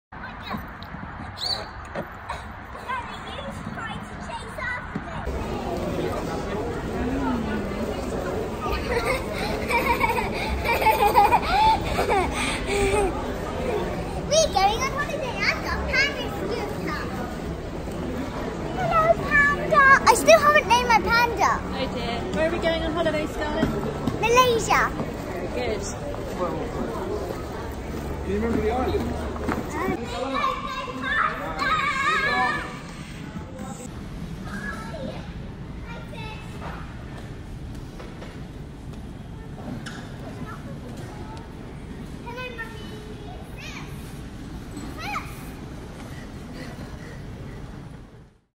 Zinc Flyte Scooter Suitcase Review
The Zinc Flyte scooter suitcase is an excellent carry on cabin bag for kids and has quite literally been our saving grace on a few long haul flights over the last few years. Investing in a scooter suitcase as kids hand luggage is one of my top travel tips for airport travel with young children.